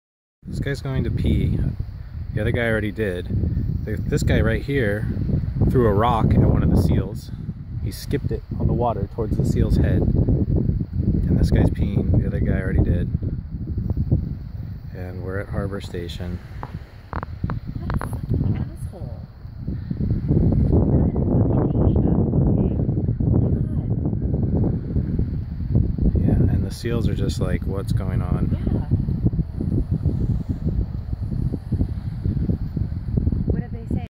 Tourists Trespassing Monterey Bay Marine Sanctuary
In September 2015 I happened to notice a kayak on a restricted beach near the Monterey Bay Aquarium and Cannery Row. Normally this beach is filled with sleeping or resting Sea Lions (mainly pups). I was staying at a house overlooking this beach for the week and the Sea Lions had never left until this day. There are "no trespassing" signs all over. I walked across the street to film the tourists so I could share it with the ranger in hopes they would be caught when returning their kayak. I cannot verify 100% they were Chinese, but they appeared to be and the majority of asian tourists in that area are Chinese. They both pissed on the beach, then one started skipping rocks towards the frightened and confused pups. He stopped when he noticed I was filming him, then started looking at his phone. Eventually a local noticed them and he yelled so loudly at them it scared them away. I called the ranger and also sent them this video. I hope they were at the very least fined for this.